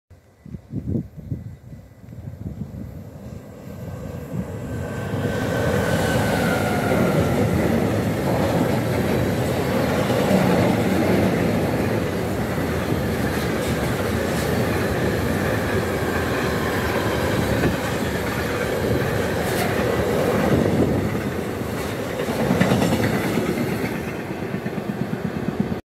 DBC 189 025 met 24 Gasketelwagens Omleidingsroute(61071/63071).
van Krabbendijke naar Kruiningen-Yerseke, onderweg naar Sloe.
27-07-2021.